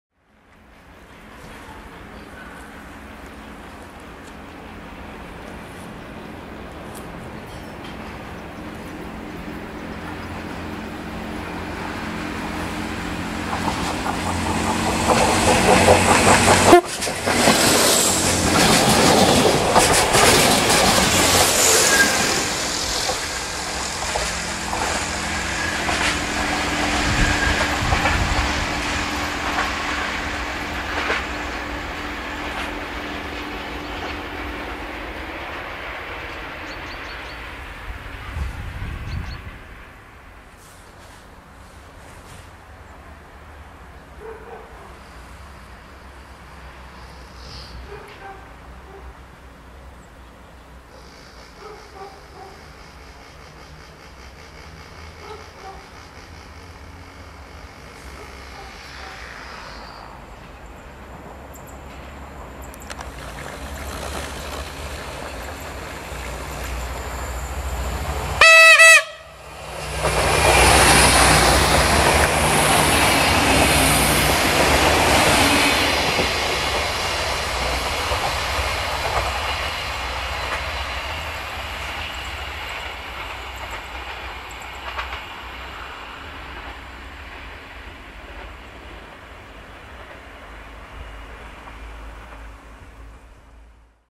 Two Railcars at Km 652+8 (12 02 2013)
Two Railcars at Km 652+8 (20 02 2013)
Doua Automotoare la Km 652+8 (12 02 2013)

1:R15206 - Oradea - Cluj Napoca - TFC - VT614
2:R3076 - Oradea - Cluj Napoca - Desiro
3:Video Ending

© Train97